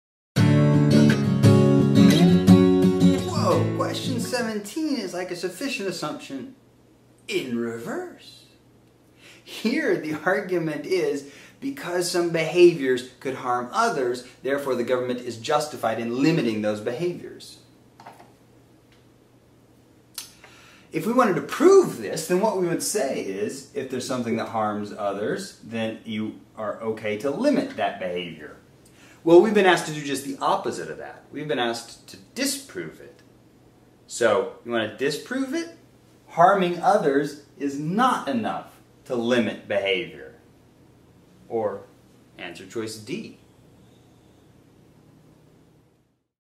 PrepTest 72, Section 3, Question 17, LSAT Prep with Dave Hall of Velocity Test Prep
Dave Hall explains right answers from the Logical Reasoning section from the June 2014 LSAT.